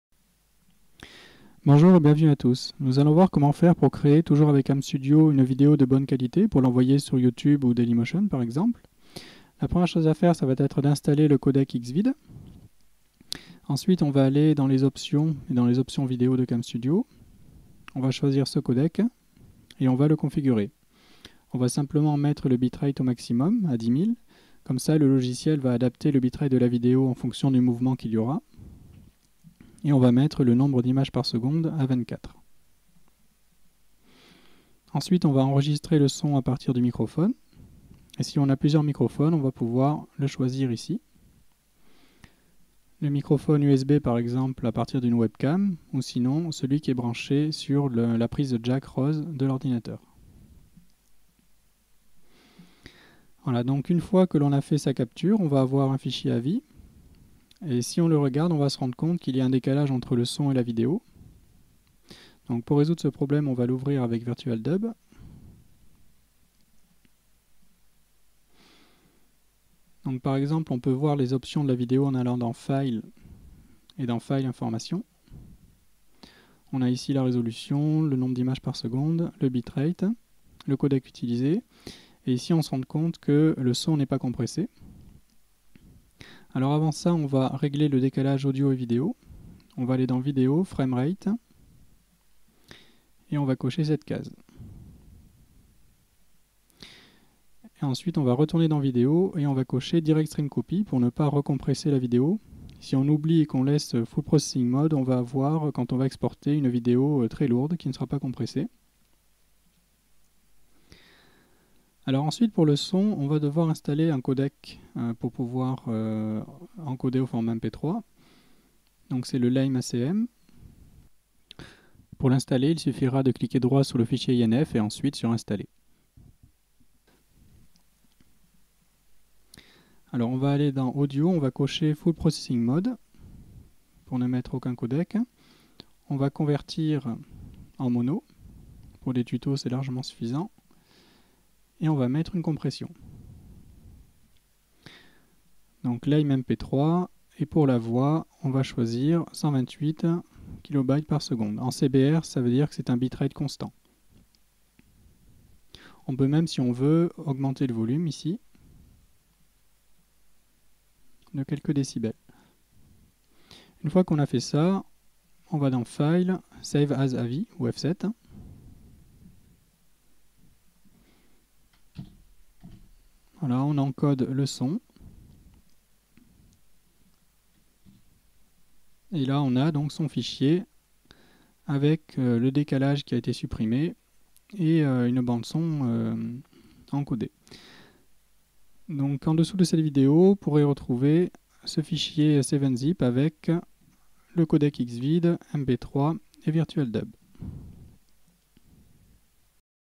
Vidéo prête à poster avec camstudio
Comment enregistrer une vidéo déjà encodé avec Xvid, résoudre le problème de décalage sonore et compresser l'audio.